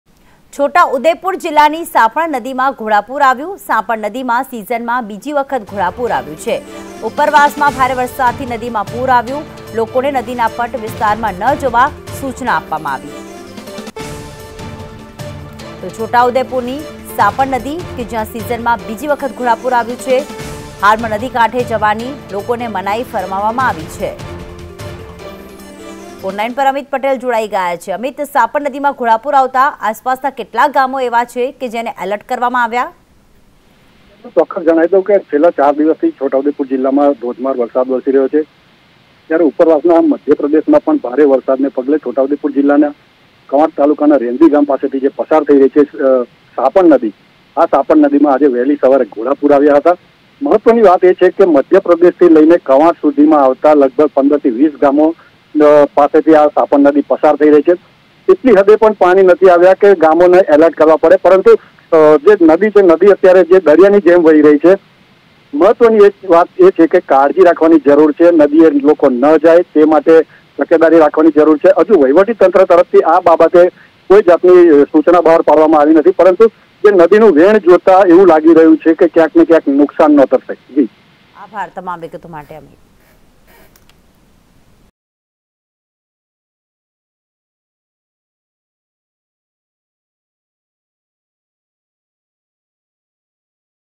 Chhota Udepur Rain | છોટા ઉદેપુર જિલ્લાની સાપણ નદીમાં ઘોડાપૂર | Monsoon | Flood | Heavy Rain
છોટા ઉદેપુર જિલ્લાની સાપણ નદીમાં ઘોડાપૂર
સાપણ નદીમાં સીઝનમાં બીજી વખત ઘોડાપૂર
ઉપરવાસમાં ભારે વરસાદથી નદીમાં પૂર આવ્યું
લોકોને નદીના પટ વિસ્તારમાં ન જવા સૂચના